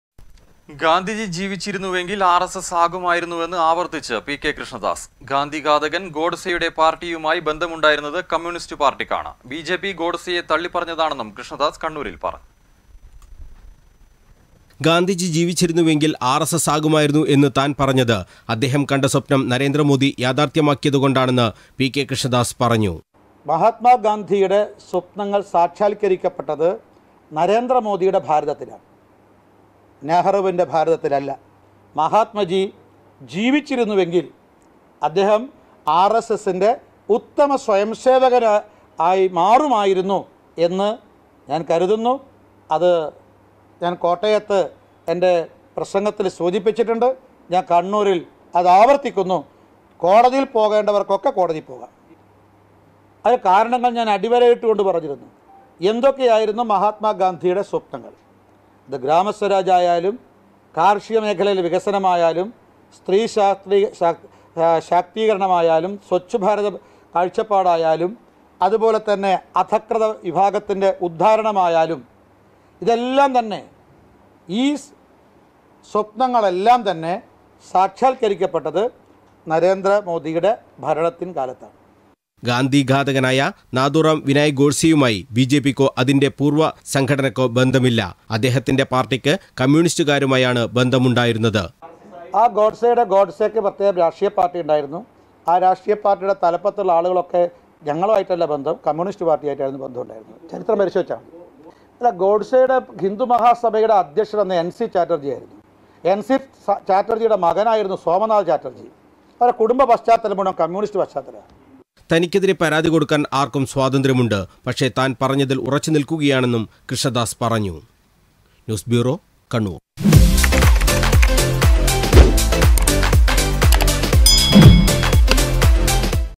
ഗോഡ്സെക്ക് ബന്ധം കമ്യൂണിസ്റ്റ് പാർട്ടിയുമായി.
ഗോഡ്സെക്ക് ബന്ധം കമ്യൂണിസ്റ്റ് പാർട്ടിയുമായി. ജീവിച്ചിരുന്നെങ്കിൽ ഗാന്ധിജി ആർ എസ് എസ് ആകുമായിരുന്നു -പി കെ കൃഷ്ണദാസ്